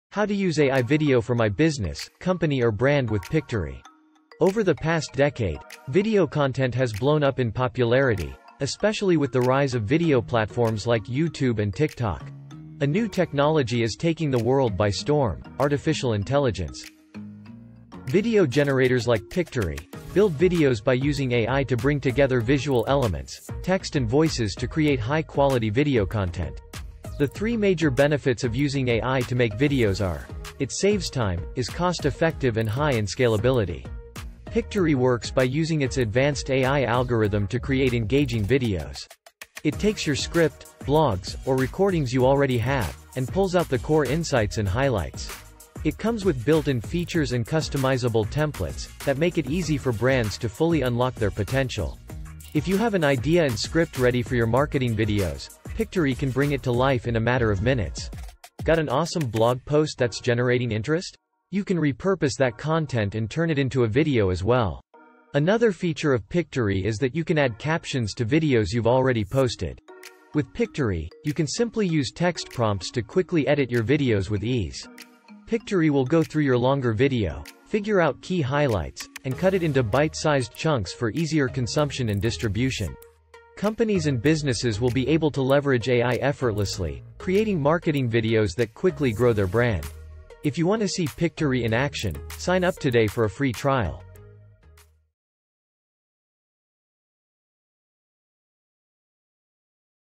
How to Use AI Video for My Business, Company or Brand with Pictory
Watch this video to find out everything you need to know about AI generators and how you can use Pictory for your business, company or brand!

Made with Pictory.ai
Sign up for your FREE Pictory account at